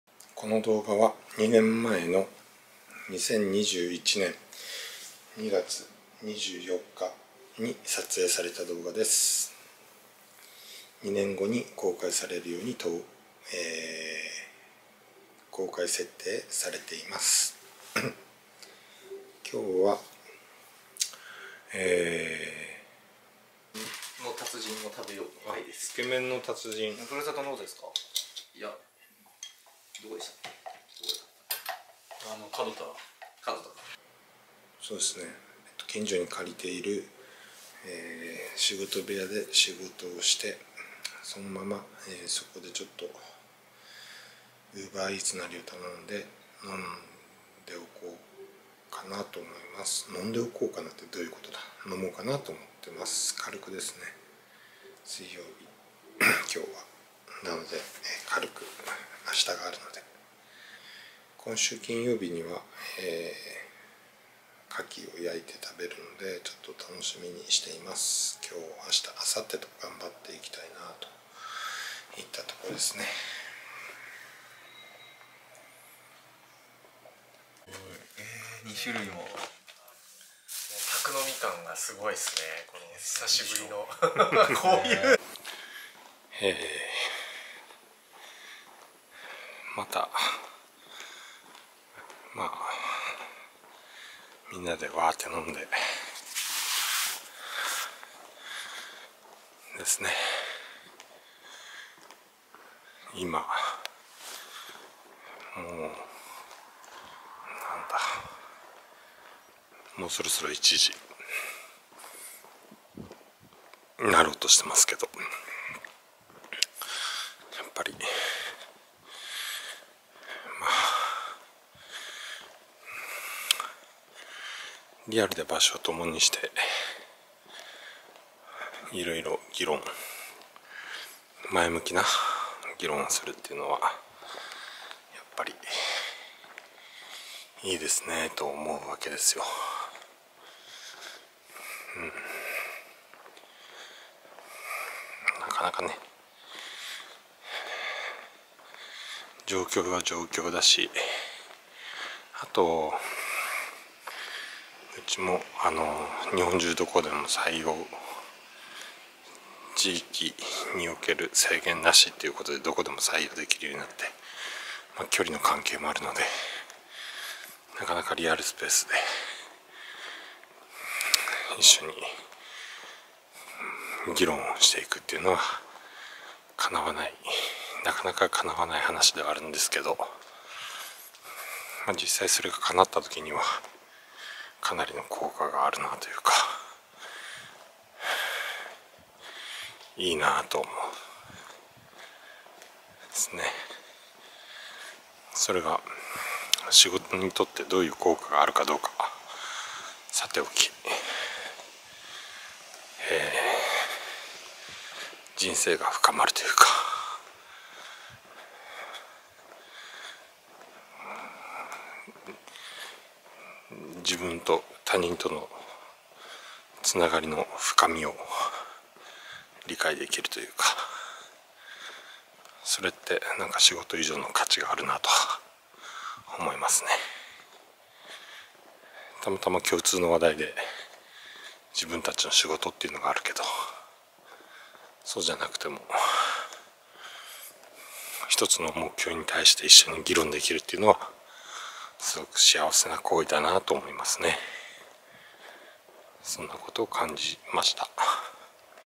Feb. 24, 2021 仕事部屋にはテーブルがない / Masa's Vlog
This daily vlog was taken 2 years ago, on Feb. 24, 2021.
We had a drinking party at my privately rented workroom. I ordered a steak from Uber Eats, but there was no table in the workroom.
I was talking about something in the middle of the night, but I don't understand what I was talking about.

この日常のVlogは、2年前の2021年2月24日に撮影したものです。
個人的に借りている仕事部屋で飲み会をしました。Uber Eatsでステーキを注文しましたが、仕事部屋にはテーブルがありませんでした。
夜中に何かを話していたのですが、何を話していたのかわかりません。